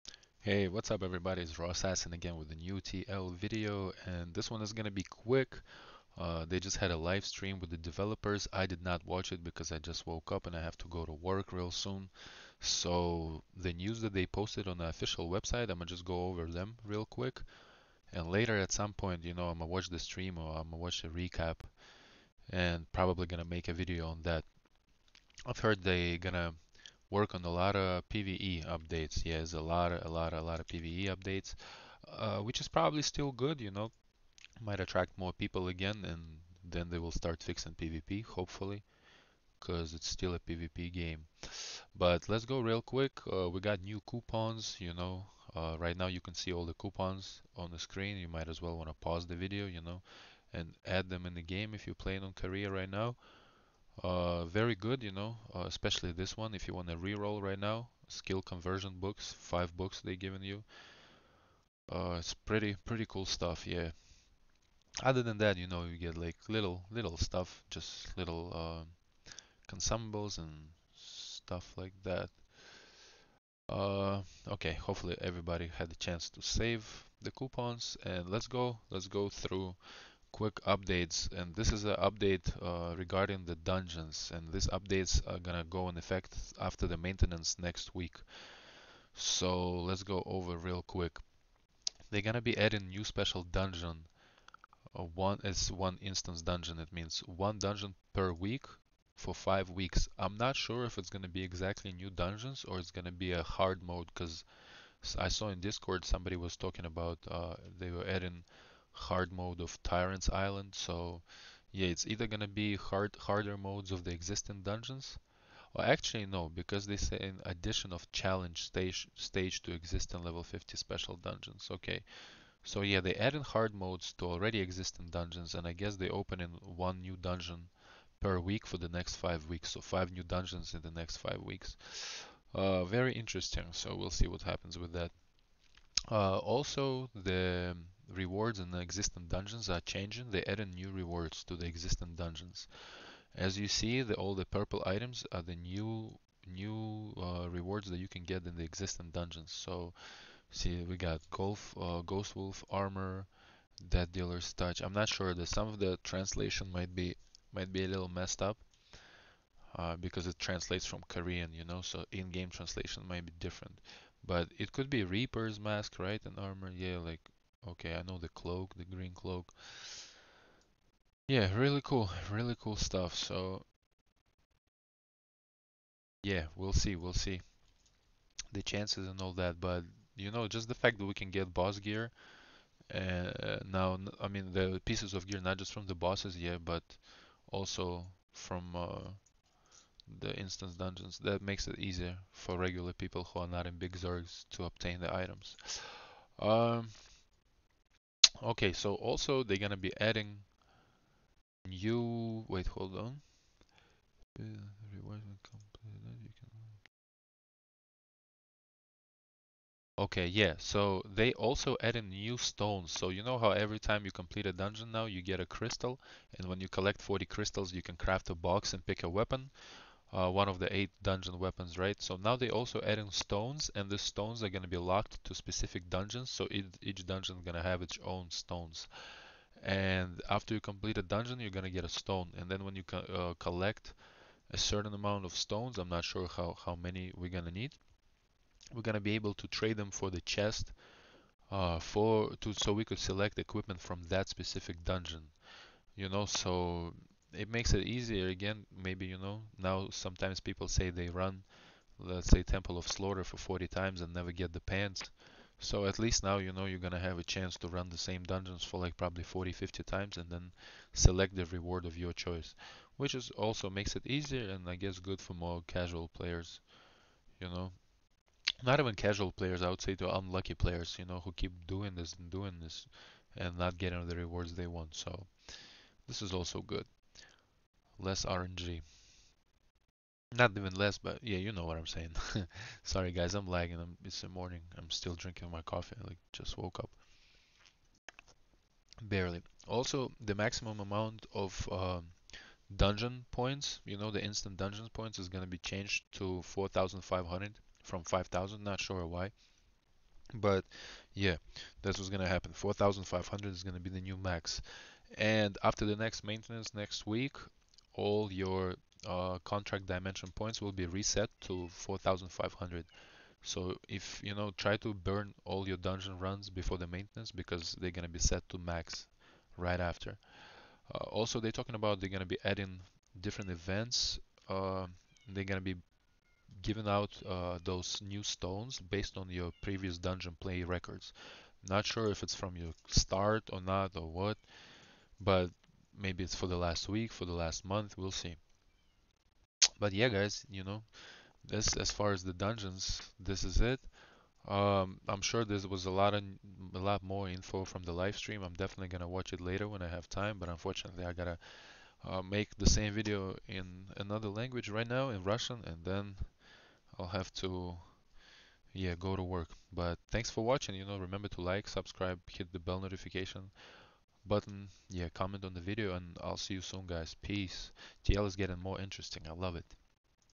Throne And Liberty. Scheduled Update For March 13th
I haven't watched the livestream with the devs yet guys, gotta go to work! But here are some of the news that will go in effect next week! Enjoy!